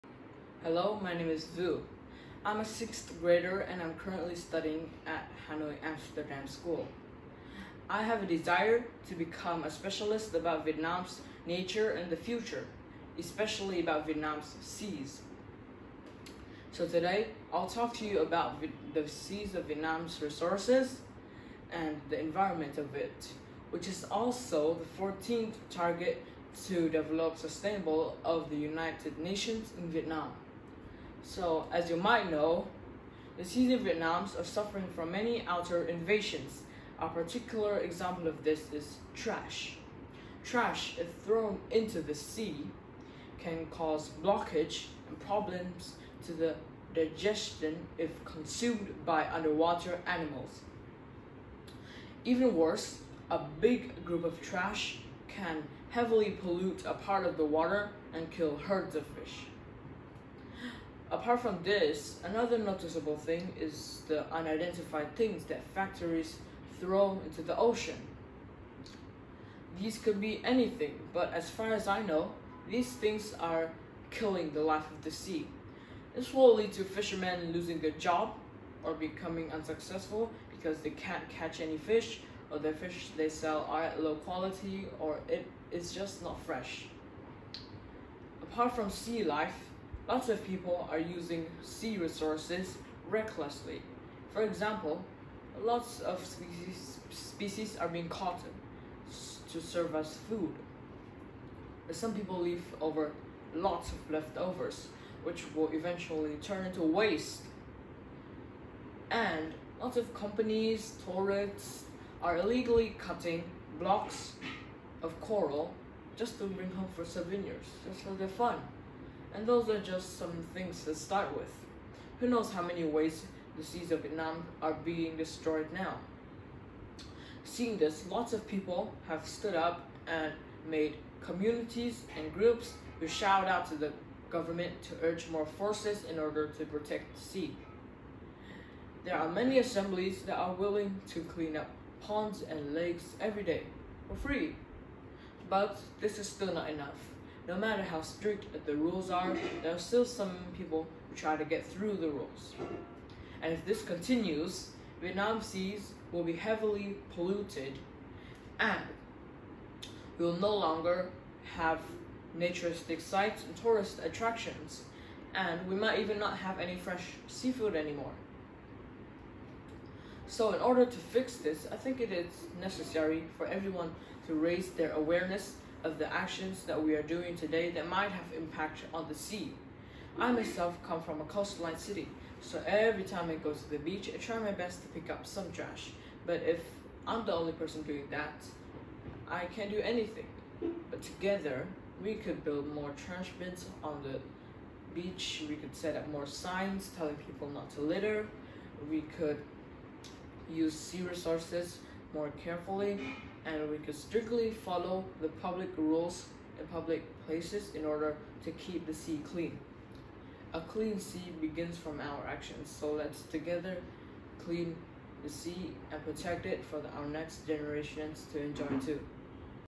TNTA - Phan Vũ (lớp 6E, trường PTCS Amsterdam Hà Nội , Nam Từ Liêm , TP. Hà Nội).
Mời quý nhà trường, quý phụ huynh và các bạn nhỏ đón xem clip dự thi của thí sinh: Phan Vũ (lớp 6E, trường PTCS Amsterdam Hà Nội , Nam Từ Liêm , TP. Hà Nội).